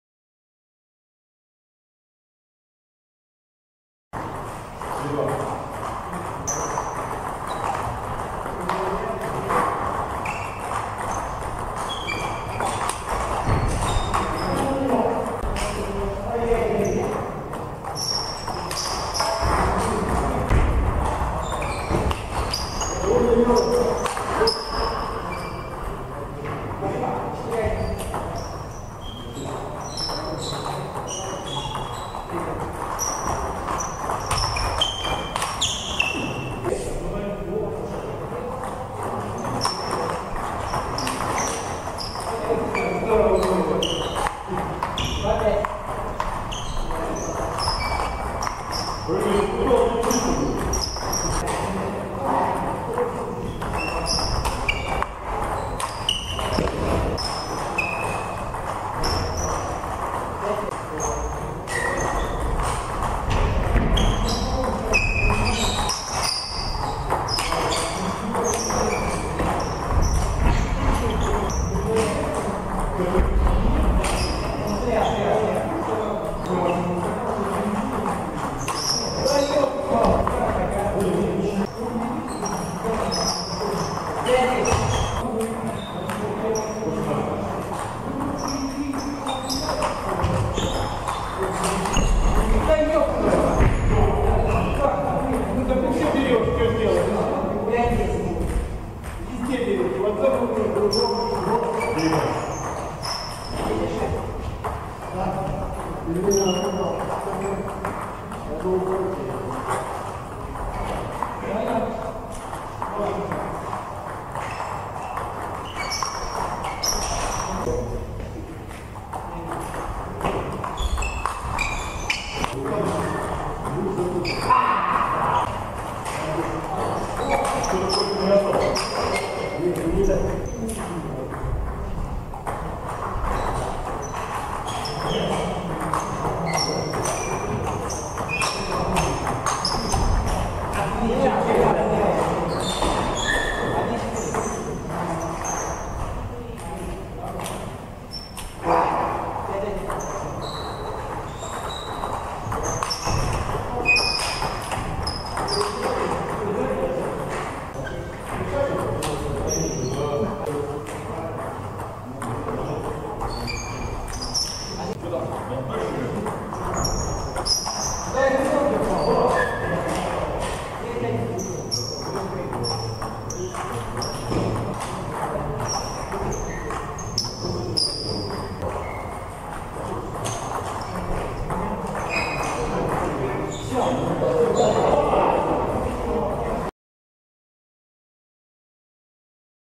table tennis Товпышко-Чирков 13.10.11
один из матчей любительского турнира на Перекопской, Санкт-Петербург